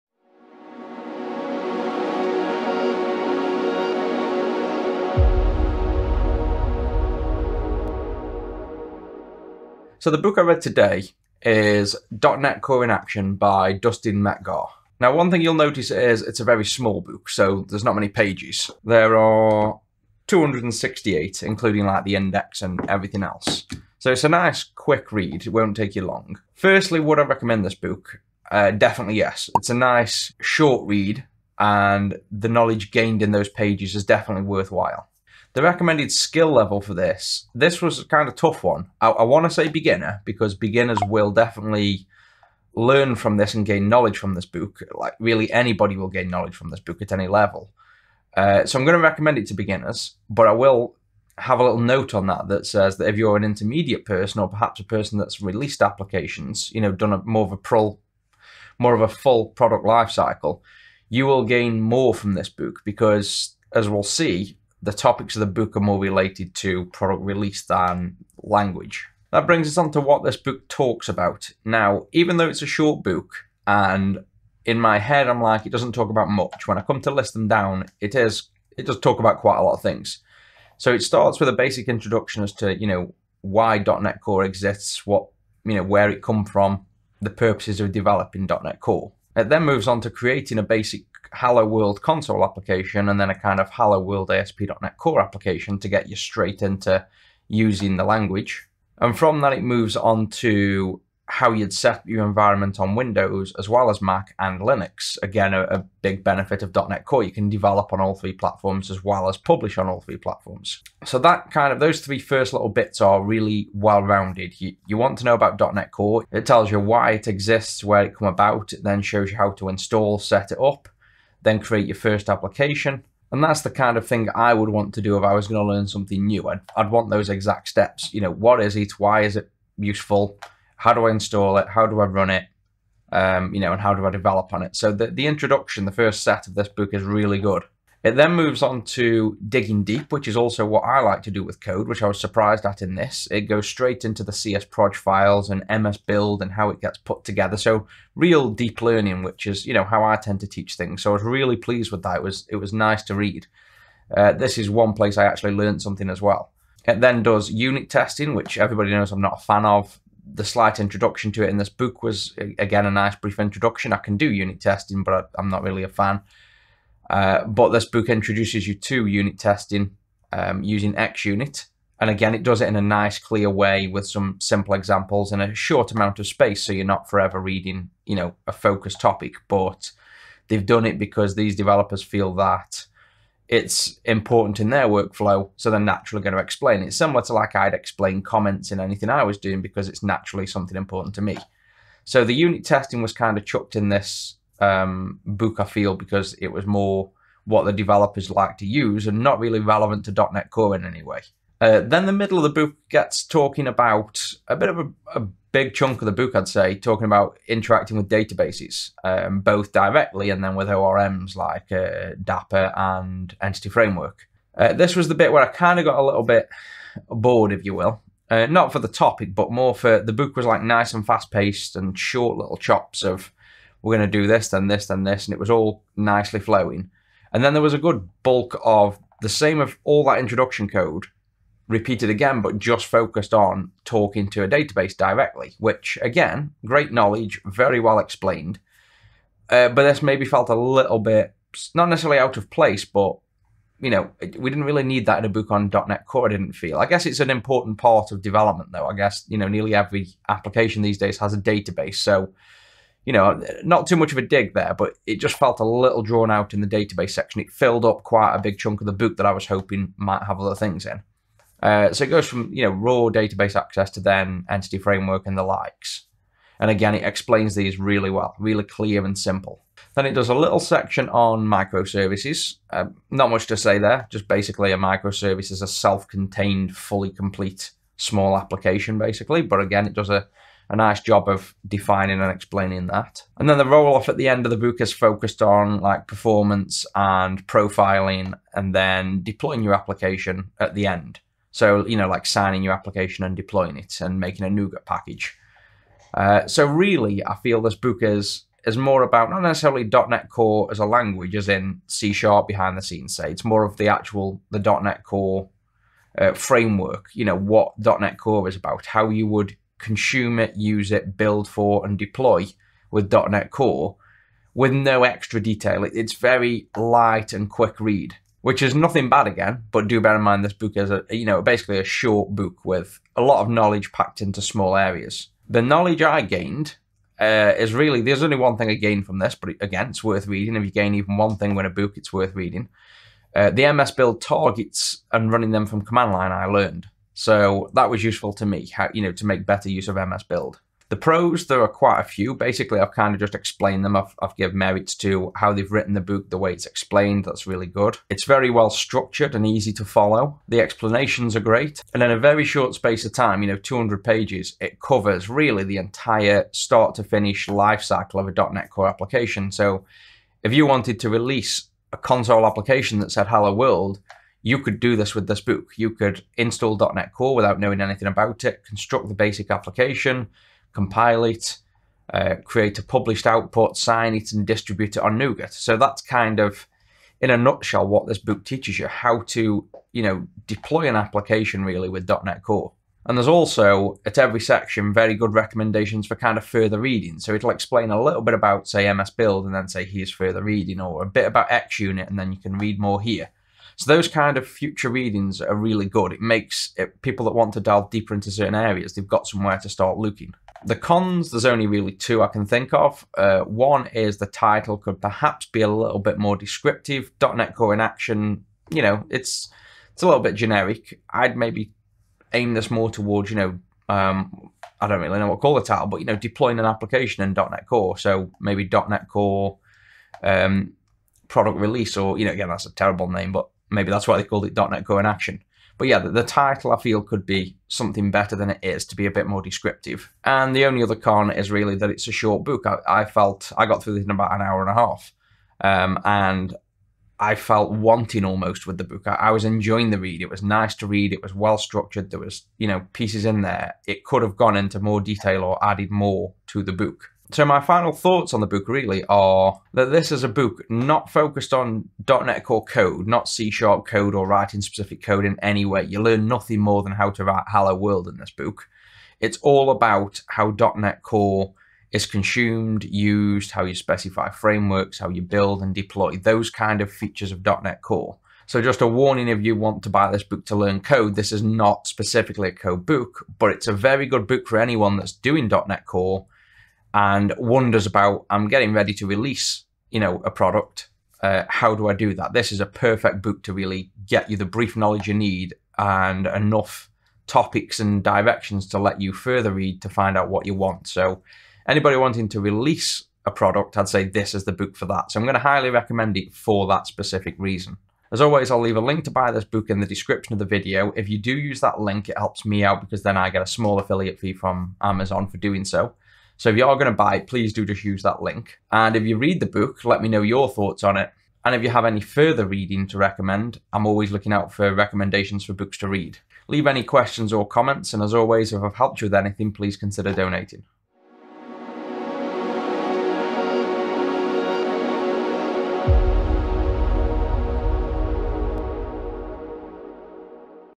AngelSix Reads .Net Core In Action by Dustin Metzgar
Would I recommend
 - Yes

Recommended Skill Level
 - Beginner

Talks about
 - General overview of where and why .Net Core came about
 - Installing, simple hello world then deployment
 - Developing on Windows, Mac and Linux
 - MSBuild
 - Unit Testing
 - Databases and ORMs
 - Microservices
 - Performance and profile
 - Deploying an application

Knowledge Gained
 - Few useful tips of using MSBuild in unique ways

Pros
 - Very nicely structured
 - Easy to follow, read and understand
 - Covers almost an entire product release from start to release
 - Recommends good further reading per section

Cons
 - Very short book, could of been a little more feature packed
 - Better book title to describe it

Final Thoughts
 - Its a book more about how to work with and deploy apps with .Net Core and no real coding. Nothing wrong with that, but to be noted this book is really for those wondering how to use and consume .Net Core for production and is more suited to those wanting to move from Framework to Core